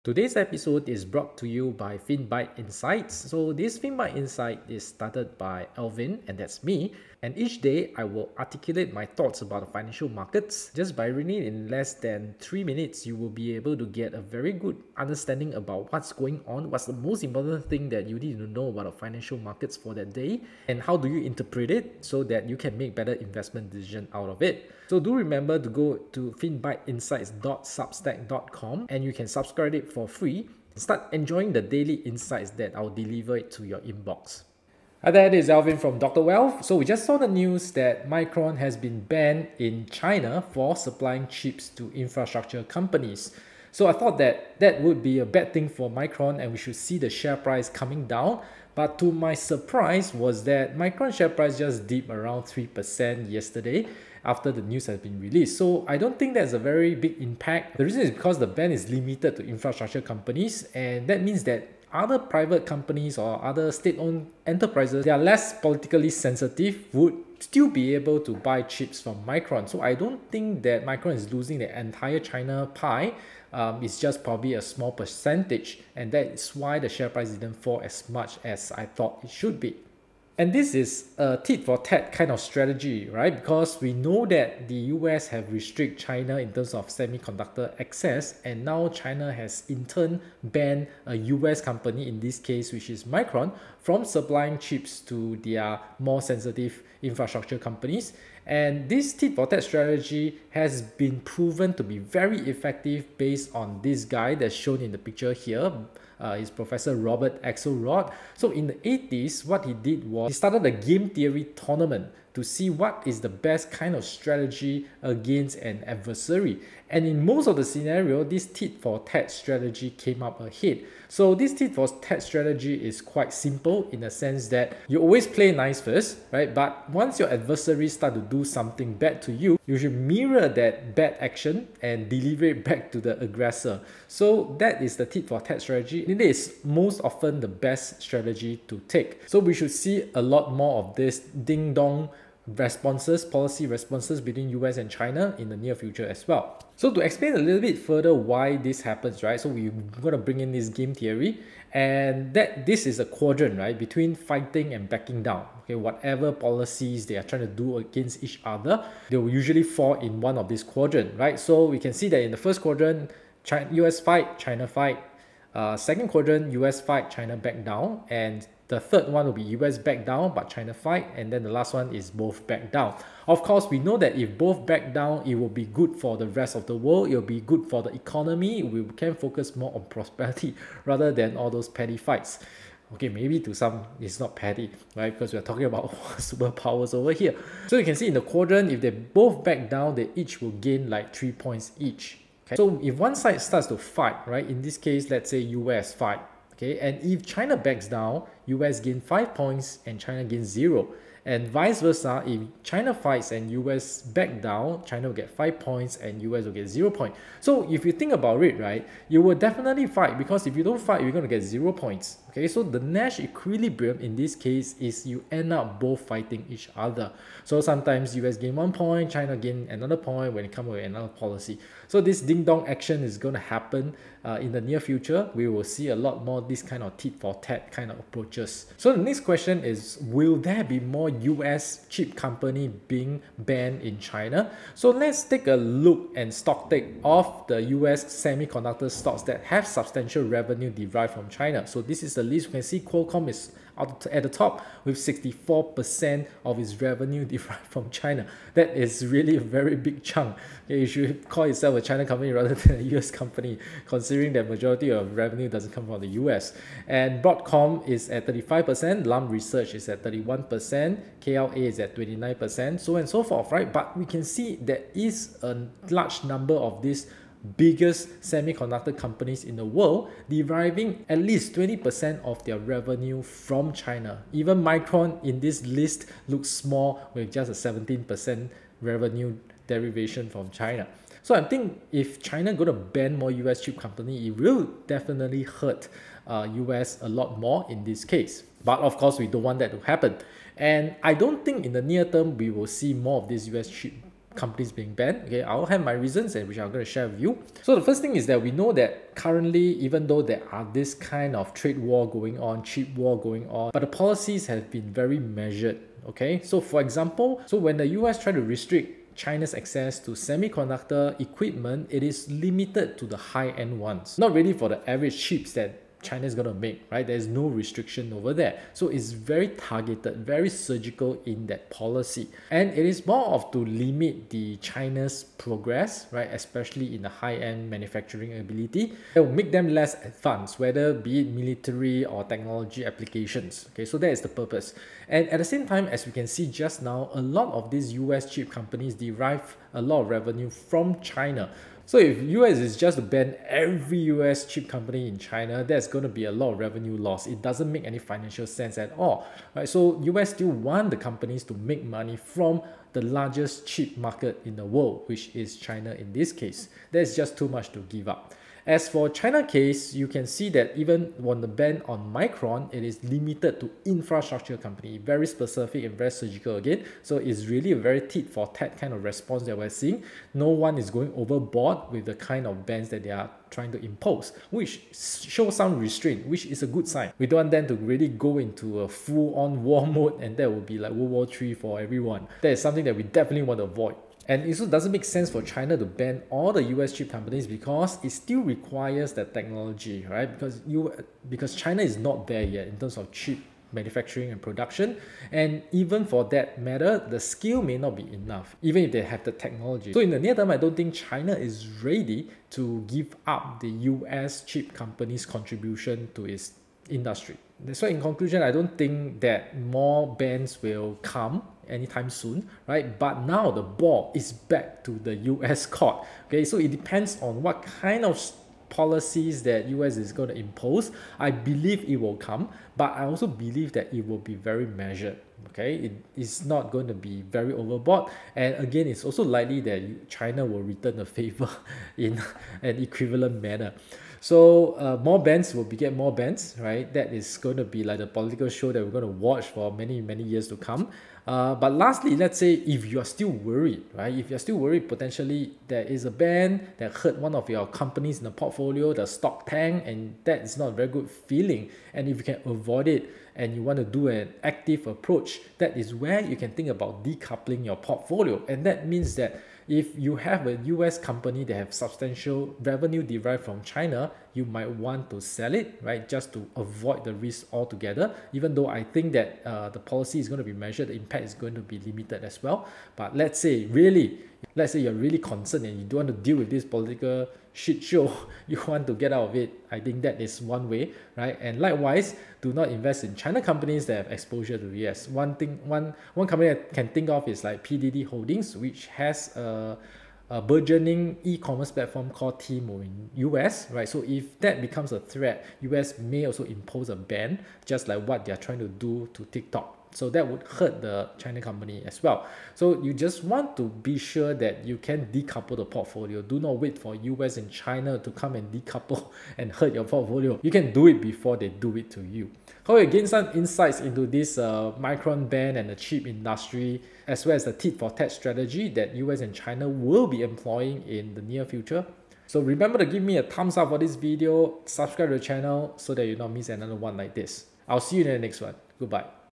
Micron Banned, More to Come?
China recently block operators of key infrastructure from buying chips from Micron. Find out the impact and how many more US chip companies derive big revenue from China. What should you do if you are concerned?

PS: Yes, the focus box was not turned off so you will see it in the whole video. :P